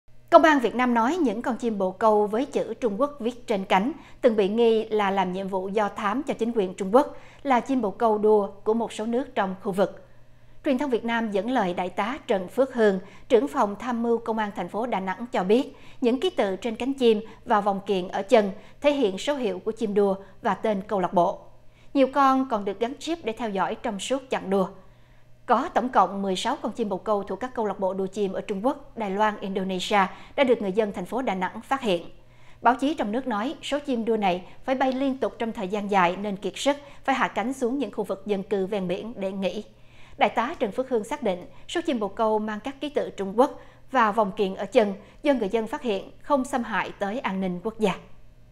Chim bồ câu mang ký tự Trung Quốc là ‘chim đua’
Tin tức Nếu không vào được VOA, xin các bạn hãy vào để vượt tường lửa. Cảnh sát Việt Nam nói những con chim bồ câu với chữ Trung Quốc viết trên cánh, từng bị nghi ngờ làm nhiệm vụ do thám cho chính quyền Trung Quốc, là chim bồ câu đua của một số nước trong khu vực. Truyền thông Việt Nam dẫn lời Đại tá Trần Phước Hương, Trưởng phòng tham mưu Công an thành phố Đà Nẵng cho biết những ký tự trên cánh chim và vòng kiềng ở chân thể hiện số hiệu của chim đua và tên câu lạc bộ. Nhiều con còn được gắn chip để theo dõi trong suốt chặng đua.